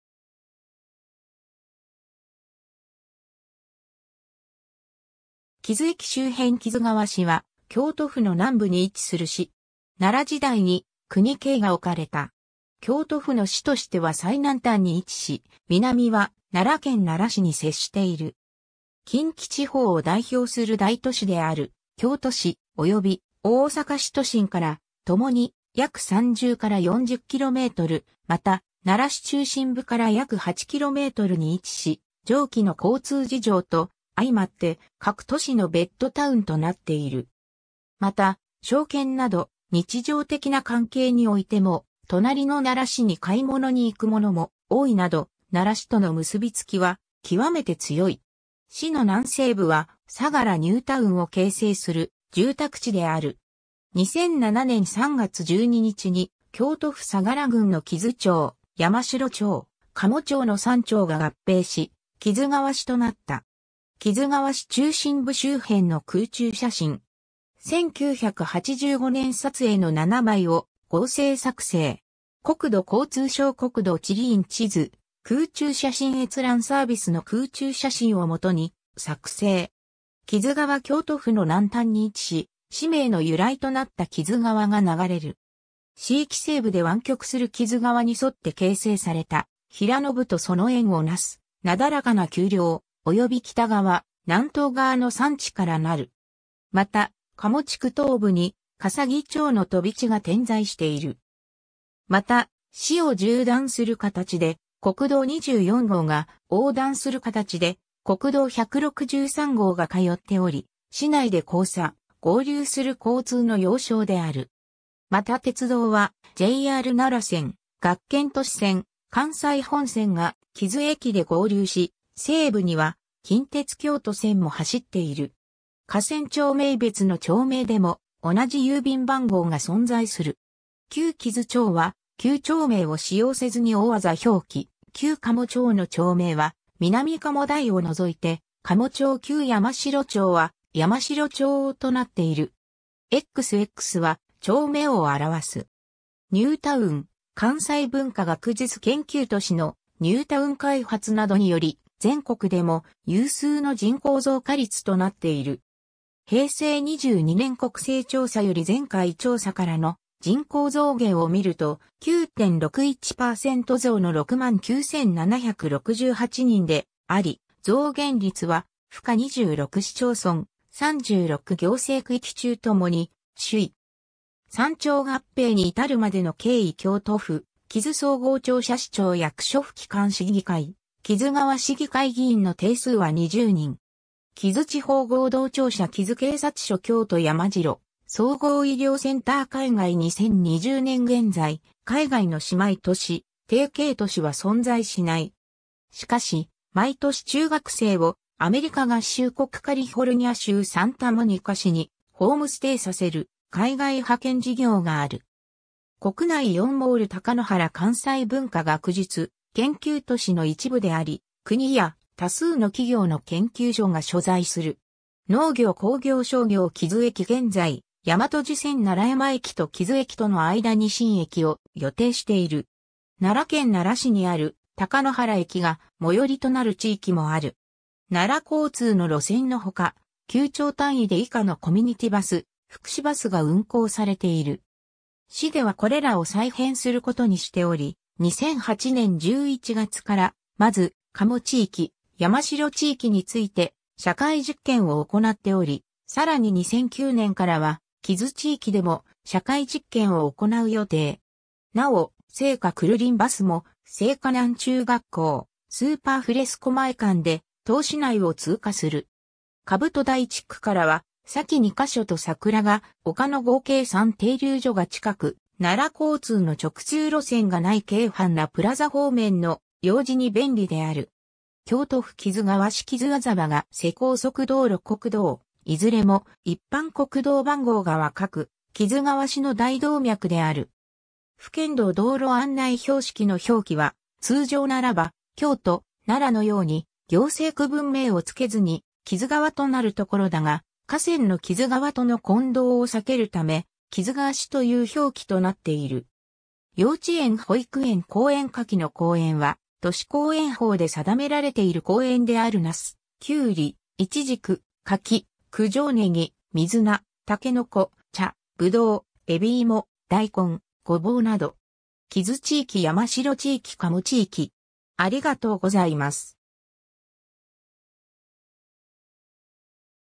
木津川市
木津駅周辺 木津川市（きづがわし）は、京都府の南部に位置する市。
 奈良時代に恭仁京が置かれた。
京都府の市としては最南端に位置し、南は奈良県奈良市に接している。
 近畿地方を代表する大都市である京都市、および大阪市都心から、ともに約30〜40km、また奈良市中心部から約8kmに位置し、上記の交通事情と相俟って各都市のベッドタウンとなっている。
また、商圏など日常的な関係においても、隣の奈良市に買い物に行く者も多いなど、奈良市との結びつきは極めて強い。
市の南西部は「相楽ニュータウン」を形成する住宅地である。
 2007年（平成19年）3月12日に、京都府相楽郡の木津町、山城町、加茂町の３町が合併し、木津川市となった。
 木津川市中心部周辺の空中写真。
1985年撮影の7枚を合成作成。
 木津川 京都府の南端に位置し、市名の由来となった木津川が流れる。
 市域西部で湾曲する木津川に沿って形成された平野部とその縁をなすなだらかな丘陵、および北側、南東側の山地からなる。
また、加茂地区東部に笠置町の飛び地が点在している。
 また、市を縦断する形で国道24号が、横断する形で国道163号が通っており、市内で交差･合流する交通の要衝である。
また鉄道は、JR奈良線、学研都市線、関西本線が木津駅で合流し、西部には近鉄京都線も走っている。
 河川 町名 別の町名でも同じ郵便番号が存在する。
 旧木津町は旧町名を使用せずに大字表記・旧加茂町の町名は南加茂台を除いて「加茂町○○」旧山城町は「山城町○○」となっている。
○○X～Xは丁目を表す。
 ニュータウン 関西文化学術研究都市のニュータウン開発などにより、全国でも有数の人口増加率となっている。
 平成22年国勢調査より前回調査からの人口増減をみると、9.61％増の69,768人であり、増減率は府下26市町村、36行政区域中ともに首位。
 三町合併に至るまでの経緯 京都府木津総合庁舎 市長 役所 府機関 市議会 木津川市議会議員の定数は20人。
 (令和元年5月17日現在) 木津地方合同庁舎 木津警察署 京都山城総合医療センター 海外 2020年現在、海外の姉妹都市･提携都市は存在しない。
 しかし、毎年中学生をアメリカ合衆国カリフォルニア州サンタモニカ市にホームステイさせる海外派遣事業がある。
 国内 イオンモール高の原 関西文化学術研究都市の一部であり、国や多数の企業の研究所が所在する。
 農業 工業 商業 木津駅 ※現在、大和路線平城山駅と木津駅との間に新駅を予定している。
 奈良県奈良市にある高の原駅が最寄りとなる地域もある。
 奈良交通の路線のほか、旧町単位で以下のコミュニティバス・福祉バスが運行されている。
 市ではこれらを再編することにしており、2008年11月からまず加茂地域・山城地域について社会実験を行っており、さらに2009年からは木津地域でも社会実験を行う予定。
 なお、精華くるりんバスも精華南中学校～スーパーフレスコ前間で当市内を通過する。
兜台地区からは左記2か所と桜が丘の合計3停留所が近く、奈良交通の直通路線が無いけいはんなプラザ方面の用事に便利である（有効本数：1日4往復）。
 京都府木津川市木津字馬ヶ瀬 高速道路 国道 ※いずれも一般国道番号が若く、木津川市の大動脈である。
 府県道 ※道路案内標識の表記は、通常ならば「京都」「奈良」のように行政区分名を付けずに「木津川」となるところだが、河川の木津川との混同を避けるため、「木津川市(Kizugawa City)」という表記となっている（徳島県吉野川市、徳島県三好市も同様）。
 幼稚園 保育園 公園 下記の公園は都市公園法で定められている公園である なす、きゅうり、いちじく、柿、九条ねぎ、みず菜、筍、茶、ぶどう、えびいも、大根、ごぼうなど。
 木津地域 山城地域 加茂地域